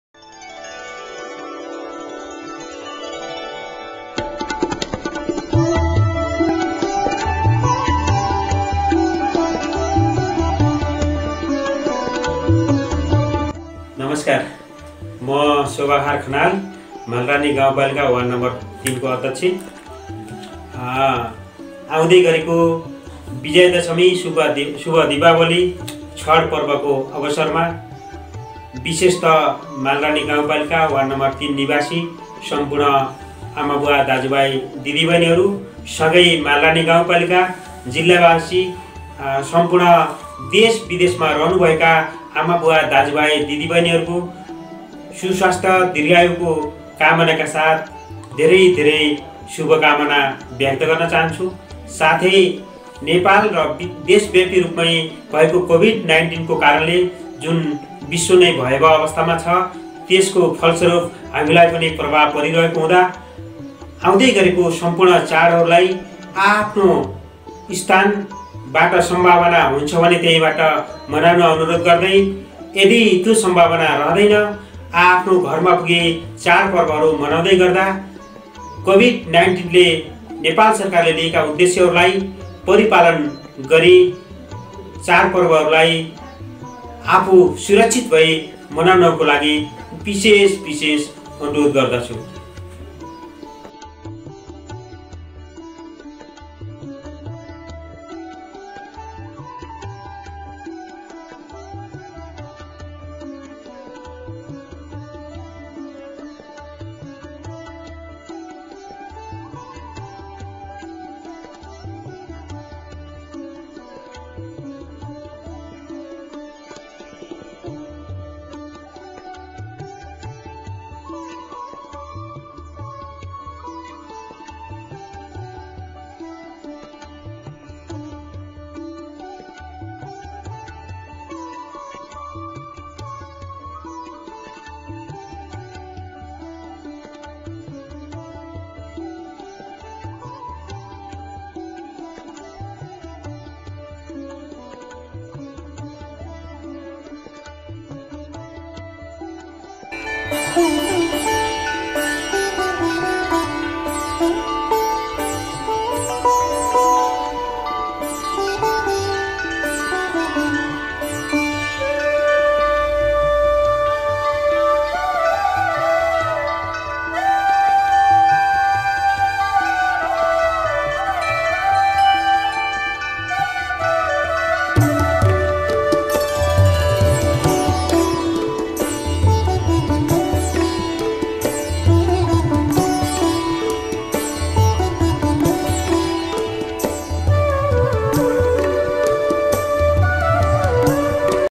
मालारानी- ३ का वडाअध्यक्ष श्री शोभाखर खनाल ज्युद्वारा दशैं, तिहार र छठको शुभकामना
मालारानी गाउँपालिका वार्ड नं- ३ खनदहका वडाअध्यक्ष  श्री शोभाखर खनाल ज्युद्वारा विजया दशमी, तिहार र छठ पर्व २०७७ को शुभकामना।

मालारानी गाउँपालिका वार्ड नं- ३ खनदहका वडाअध्यक्ष श्री शोभाखर खनाल ज्युले महान चाड शुभ विजया दशमी, तिहार र छठ पर्व २०७७ को अवसरमा सम्पूर्ण वार्डका कर्मचारी, वार्डबासी, जिल्लाबासी र देश विदेशमा रहेका समस्त नेपालीहरुमा हार्दिक शुभकामना व्यक्त गर्नुभएको छ ।  - अर्घाखाँचीडट्कम